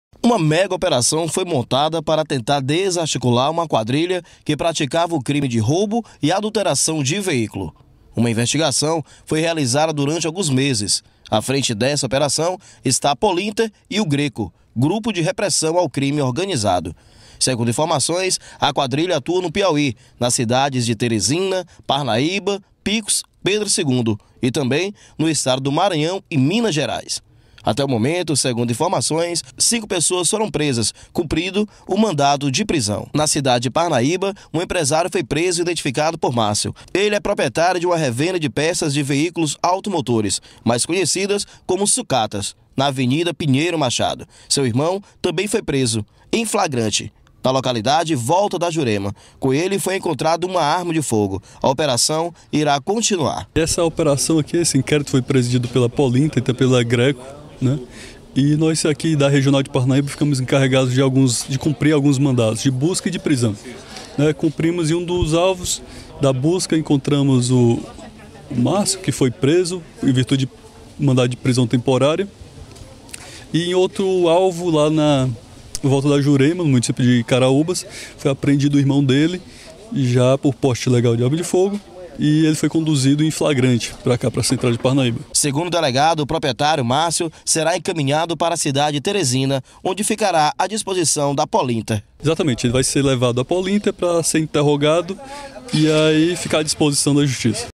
‘Adulteração’: Polícia realiza operação para prender donos de sucatas
Por volta das 05 horas da manhã desta terça-feira (30/06), a Polícia Civil do Piauí deflagrou  a ‘Operação Adulteração’, com o objetivo de desarticular uma das maiores organizações criminosas que atua no estado do Piauí.